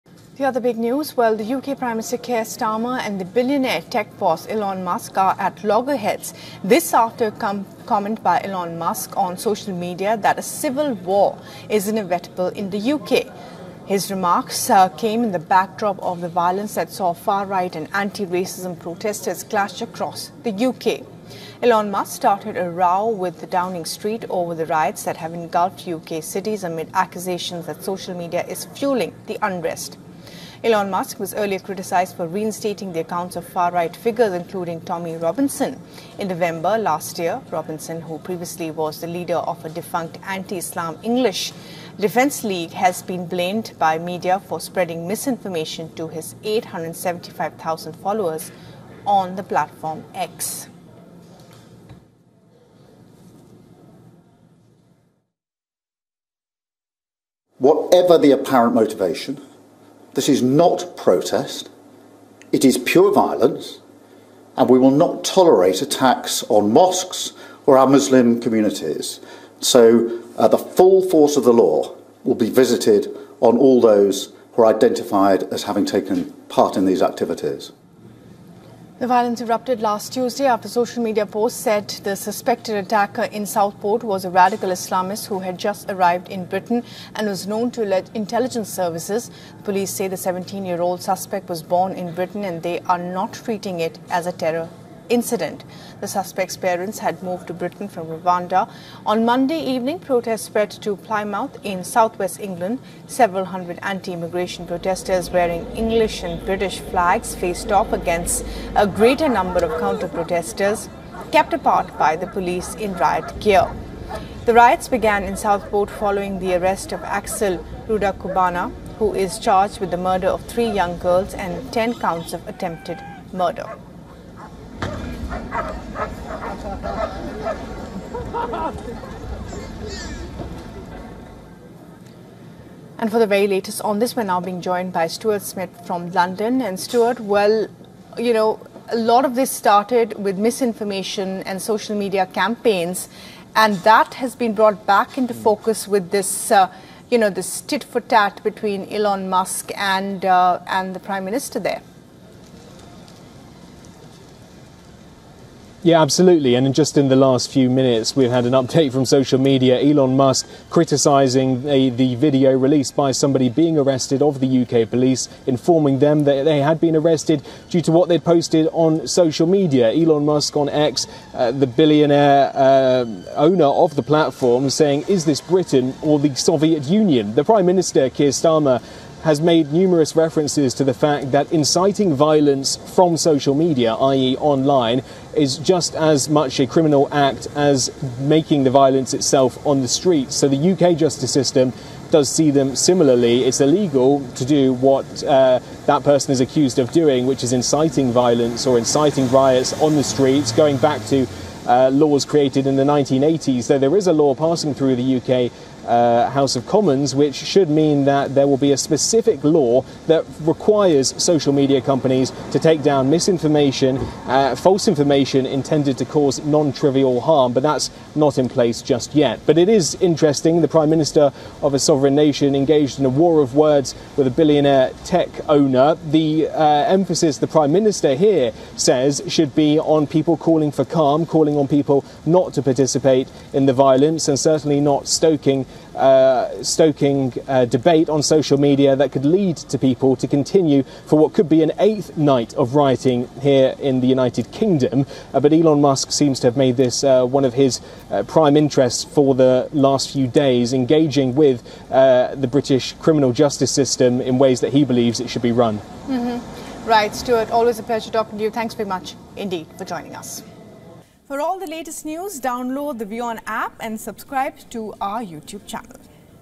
UK: Elon Musk hits back at UK PM after 'civil war' comments dismissed | WION Dispatch
UK PM Keir Starmer has condemned Elon Musk after the Tesla CEO said that "civil war is inevitable" in the UK. Musk made these comments on his social media platform X (formerly known as Twitter) in response to a video showing protesters shooting fireworks at police.